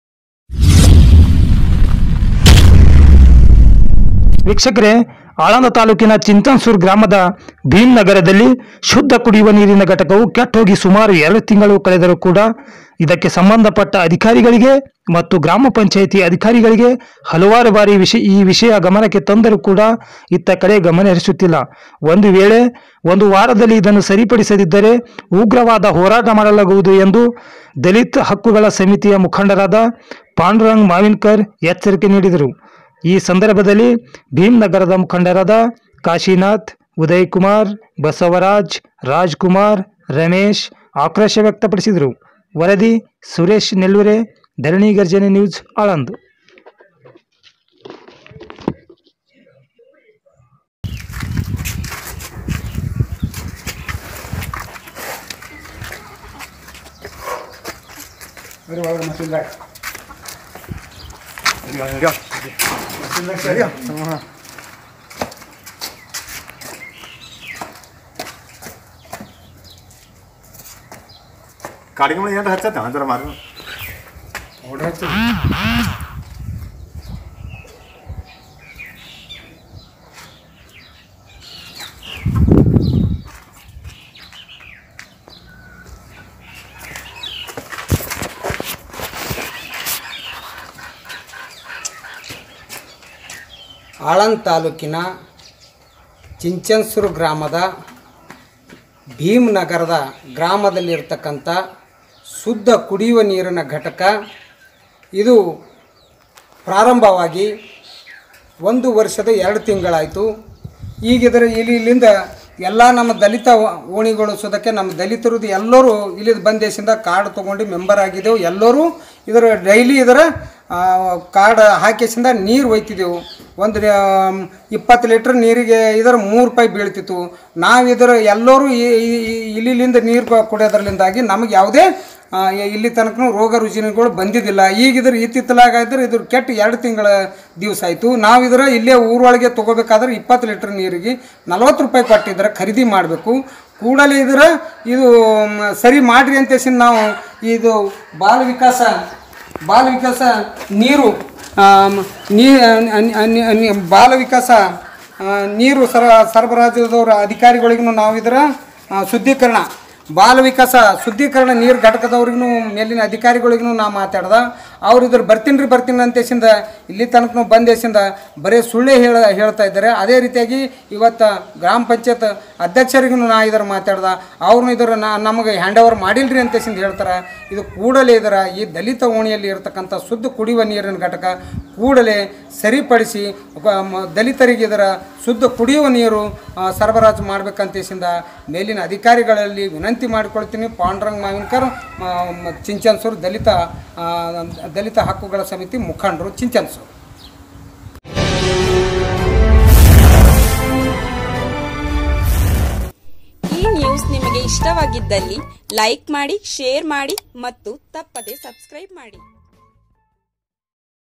ಆಳಂದ ತಾಲೂಕಿನ# ಚಿಂಚನಸುರ ಗ್ರಾಮದ# ಭೀಮ ನಗರದ ಶುದ್ಧ# ಕುಡಿಯುವ ನೀರಿನ ಘಟಕವು ಕೆಟೋಗಿ ಸುಮಾರ ಕಳೆದರು ಕೂಡಾ
ಆಳಂದ ತಾಲೂಕಿನ ಚಿಂಚನಸುರ ಗ್ರಾಮದ ಭೀಮ ನಗರದ ಶುದ್ಧ ಕುಡಿಯುವ ನೀರಿನ ಘಟಕವು ಕೆಟೋಗಿ ಸುಮಾರು 2 ತಿಂಗಳು ಕಳೆದರು ಕೂಡಾ ಇದಕ್ಕೆ ಸಂಬಂಧಿಸಿದ ಅಧಿಕಾರಿಗಳು  ಮತ್ತು ಗ್ರಾಮ ಪಂಚಾಯಿತಿ ಅಧ್ಯಕ್ಷರಿಗೆ ಹಲವಾರು ಬಾರಿ ಈ ವಿಷಯ ಗಮನಕ್ಕೆ ತಂದರೂ ಕೂಡಾ ಇತ್ತ ಕಡೆ ಗಮನ ಹರಿಸುತ್ತಿಲ್ಲ ಓಂದು ವೇಳೆ ವಾರದಲ್ಲಿ ಇದನ್ನು ಸರಿ ಪಡಿಸಿದರೆ ಉಗ್ರವಾದ ಹೋರಾಟ ಮಾಡಲಾಗುತ್ತದೆ ಎಂದು ದಲಿತ ಹಕ್ಕುಗಳ ಸಮಿತಿಯ ಮುಖಂಡರಾದ ಪಾಂಡುರಂಗ ಮಾವಿನಕರ ಎಚ್ಚರಿಕೆ ನಿಡಿದ್ರು

ವರದಿ.

ಸುರೇಶ್ ನೇಲ್ಲುರೆ
ಧರಣಿ ಘರ್ಜನೆ ನ್ಯೂಸ್ ಆಳಂದ